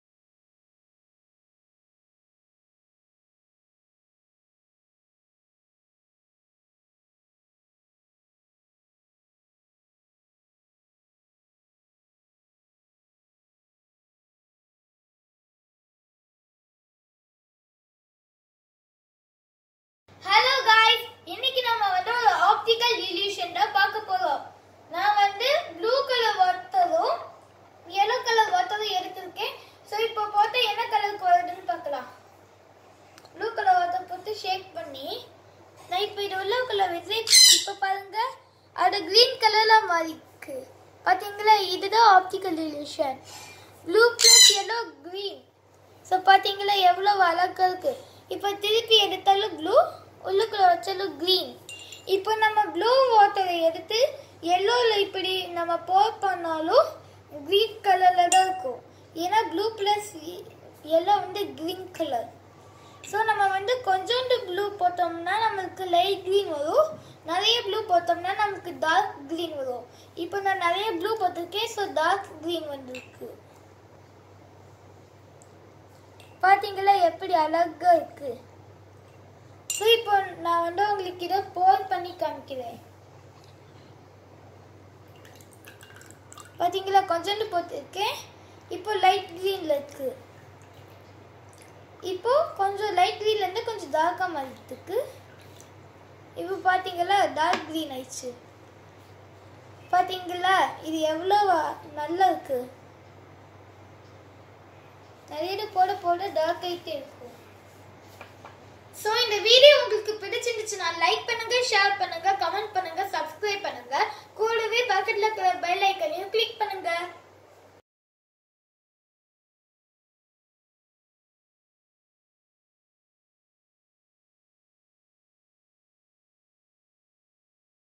Colour illusion#How to make colour illusions at home
in this video we will learn about how colour changes when we mix two colours.  water illusion how it happens